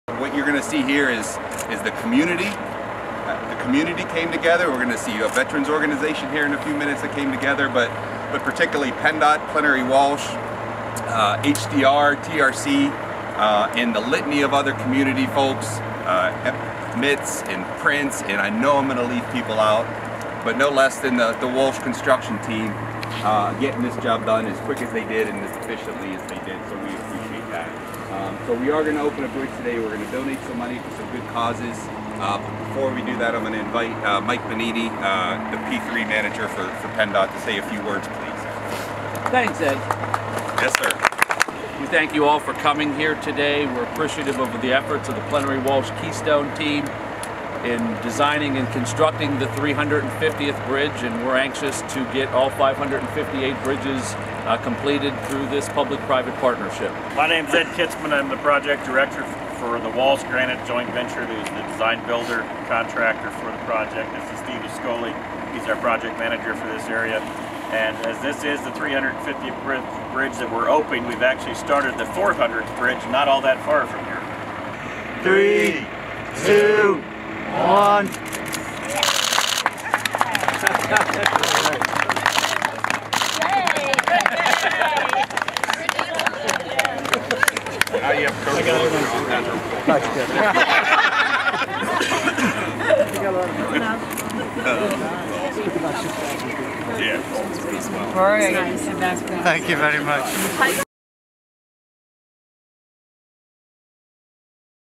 PennDOT celebrates 350th P3 Rapid Bridge Replacement project
PennDOT marks the 350th bridge replaced under the state's Rapid Bridge Replacement Project P3 public-private partnership. The bridge carries Seven Valleys Road over the south branch of Codorus Creek in Shrewsbury Township, York County, November 14, 2017. Plenary Walsh Keystone Partners is a consortium comprised of The Plenary Group, The Walsh Group, Granite Construction Company and HDR Engineering. The joint venture of Walsh Construction Company and Granite Construction Company serves as the design/build contractor on the Rapid Bridge Replacement project. The project is being procured by PennDOT as a public-private partnership.
Under the terms of the design-build contract, the Walsh/Granite joint venture is responsible for the accelerated replacement of 558 geographically dispersed, structurally deficient bridges across the Commonwealth of Pennsylvania.